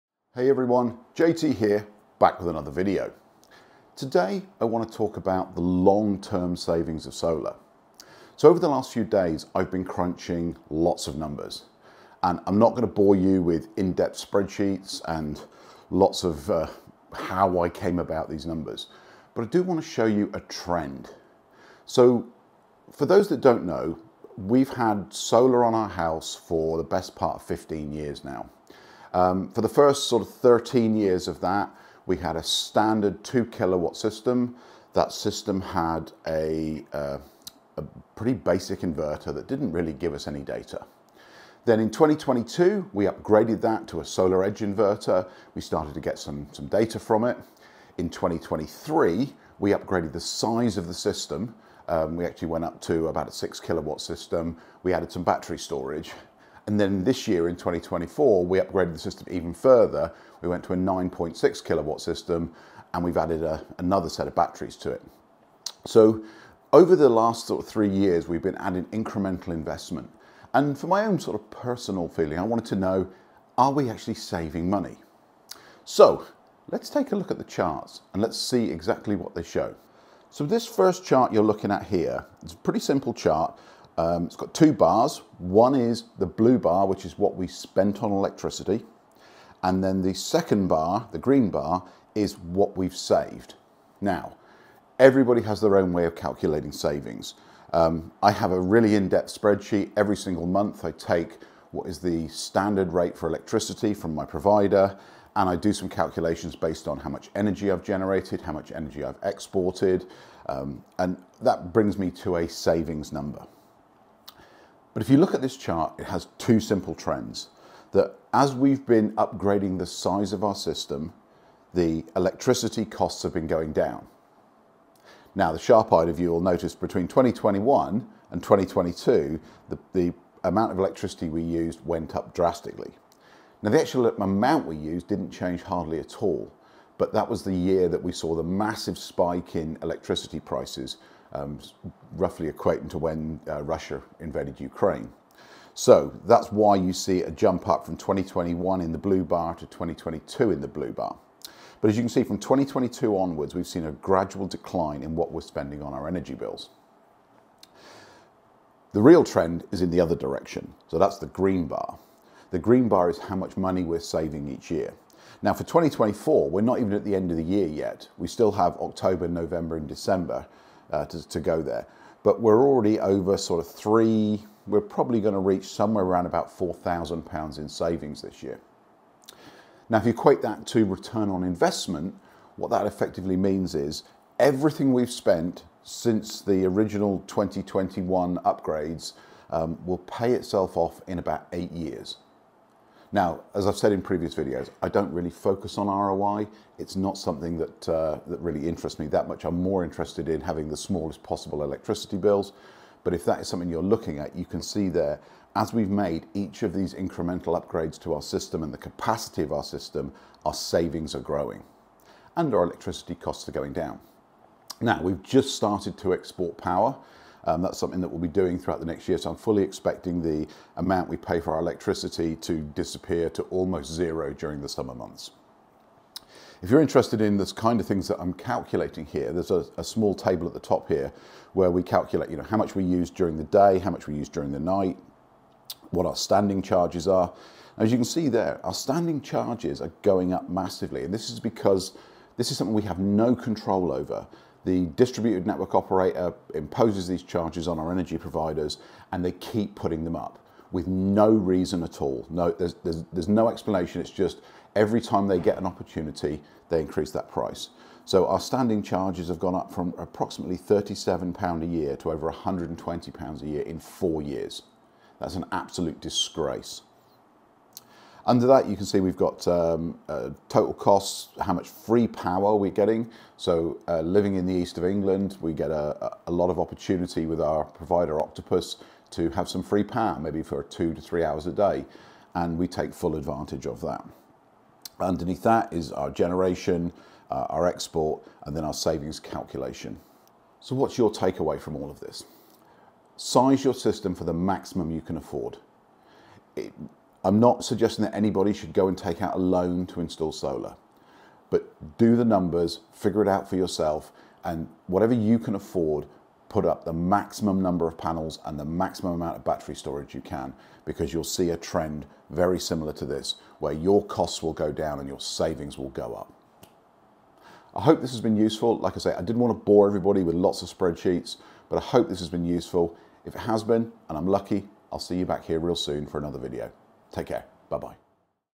Will Solar Panels REALLY Save You Money in 2025?
Yes, it does, but you probably want more than that, so let's dig in.

1️⃣Shelly EM Pro 50 - Energy Monitor
2️⃣Aqara Temperature and Humidity Sensor Kit - 3pcs
3️⃣Aqara Temperature and Humidity Sensor, Single Unit